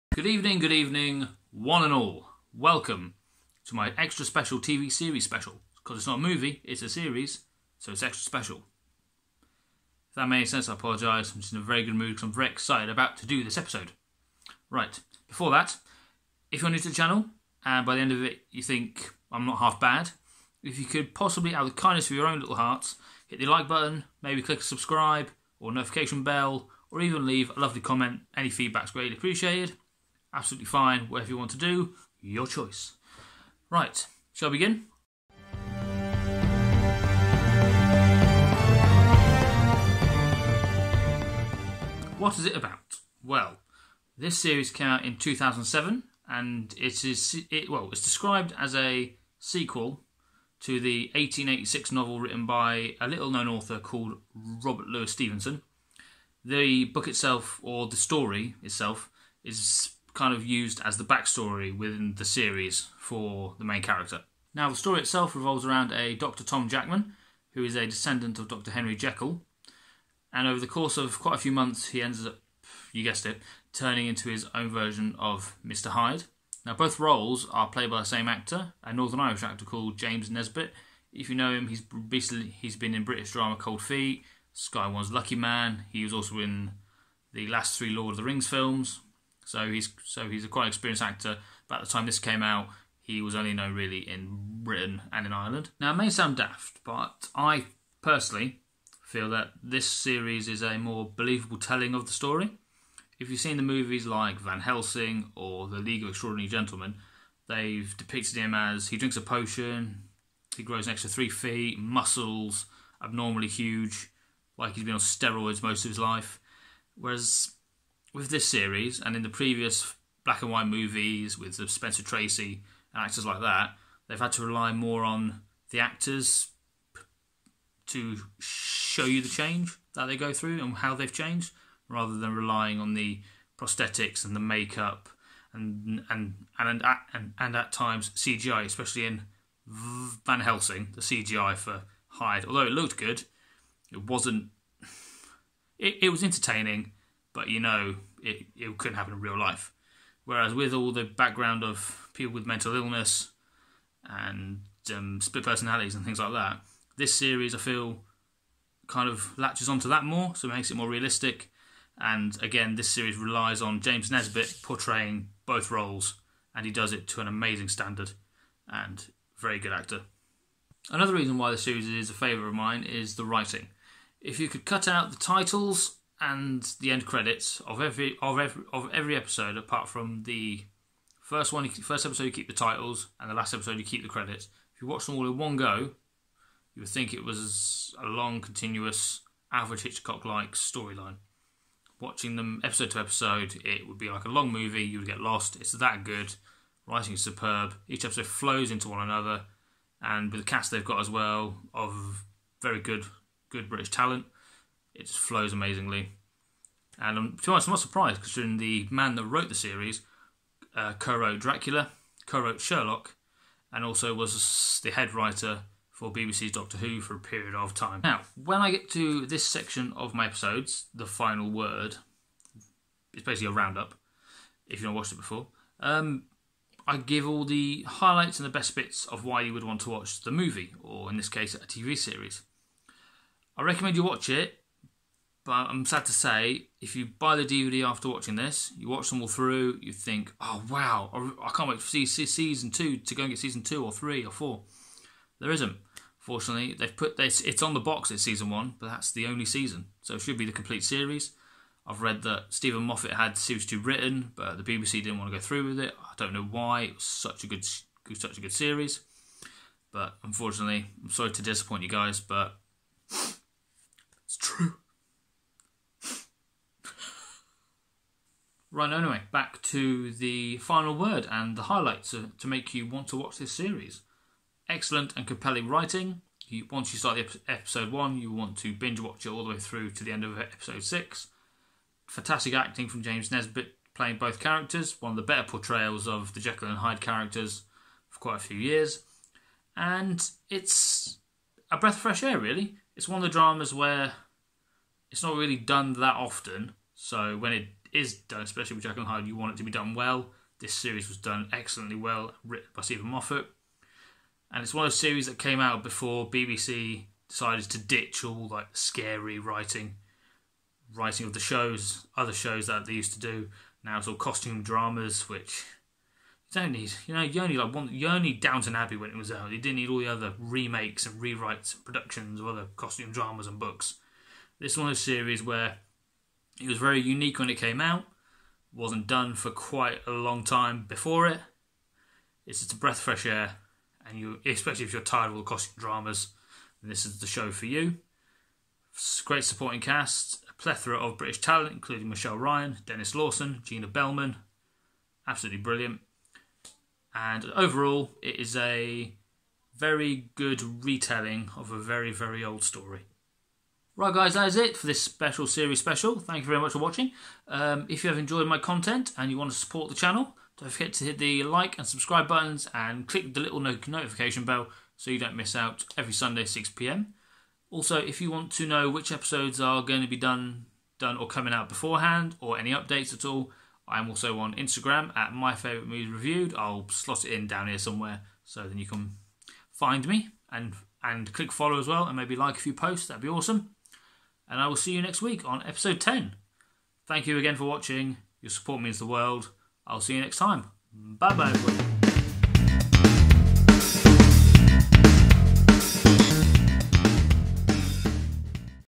UNIQUE TAKE ON A VERY OLD TALE!! | Jekyll: Series SPECIAL REVIEW (2007)
This is the 21st Century telling of Jekyll Hyde, using the 1886 novel by author Robert Louis Stevenson as an origin story for this 2007 BBC series written by well known writer Stephen Moffat.

In the series you follow Dr Tom Jackman (James Nesbitt) on his journey of struggle, dealing with his 2nd dark unknown personality (also James Nesbitt) that he's losing control off.

On top of this he must maintain a career and keep up the lies to his strong wife (Gina Bellman) while under the surveillance of a shady company intent on using Dr Jackman's 'Dark Side' to Thier advantage.

James Nesbitt (Lucky Man, Cold Feet)
Denis Lawson ( Star Wars, Holby City)
Gina Bellman (Coupling , Leverage)
Michelle Ryan (EastEnders, Bionic Woman)
Meera Syal (Goodness Gracious Me)

For my Thumbnails I use the app Polish

For my Video Editing I use the App Vlogstar